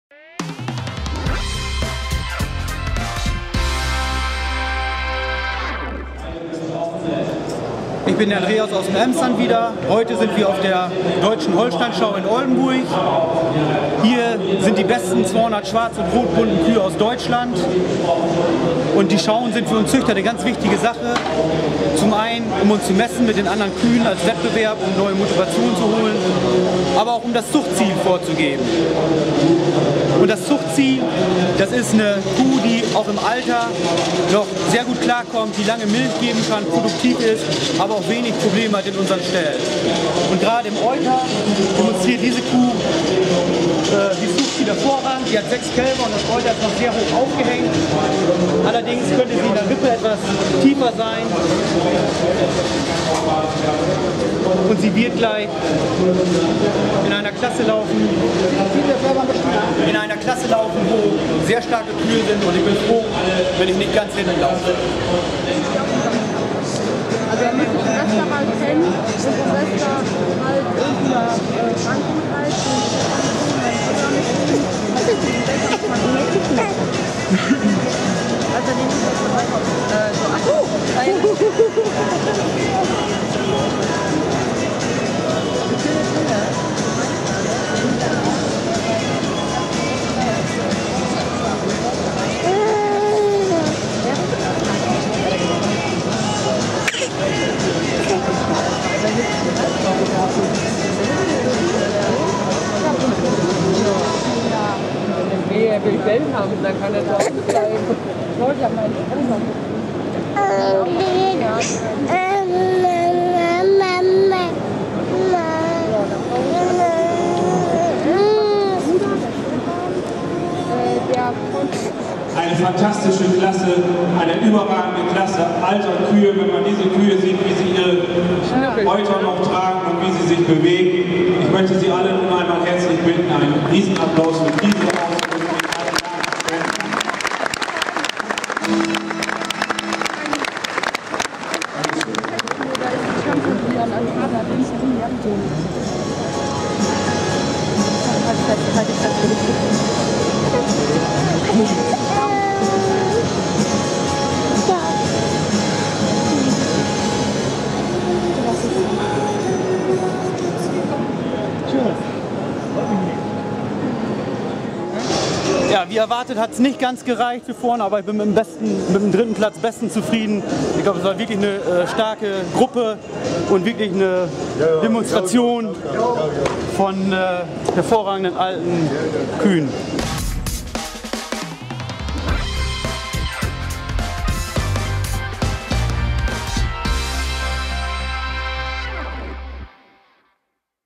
Deutsche Holstein Schau My Kuhtube Film 18
Auch bei Kühen gibt es richtig kuh-le Models. Unser My Kuhtube-Landwirt Andreas aus dem Emsland hat ein ganz besonderes Model bei sich zu Hause im Stall und das will er heute allen zeigen!
Wie es ausgeht und welchen Platz er belegt, seht ihr jetzt selbst!